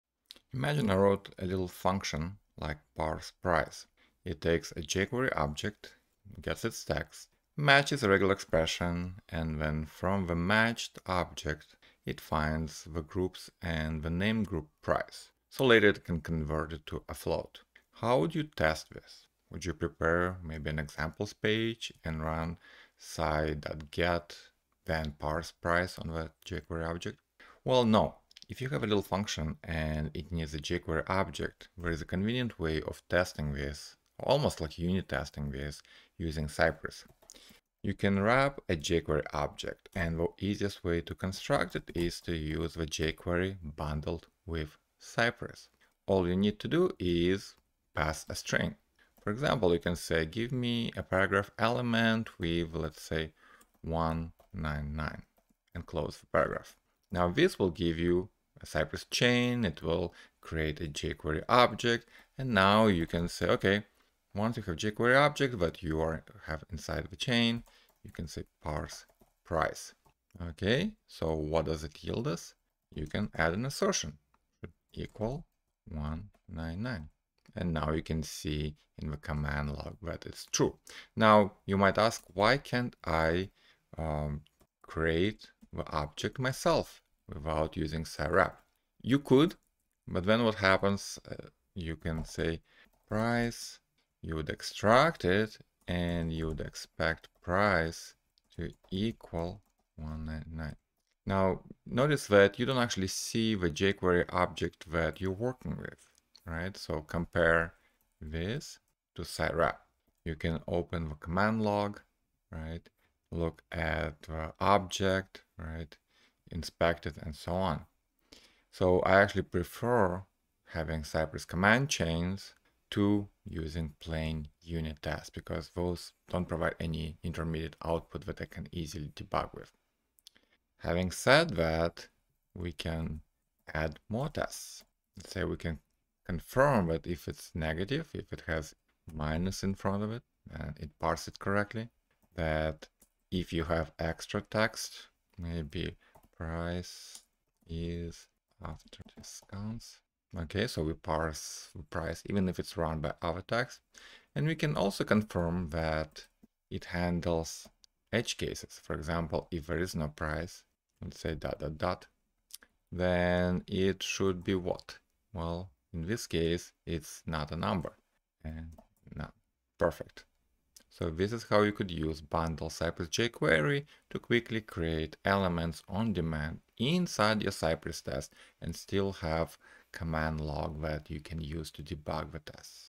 How To Unit Test Find And Parse The Price Function That Needs A jQuery Object
If you need to unit test a function that expects a jQuery object as its argument, you can quickly create such objects using Cypress.$ jQuery library.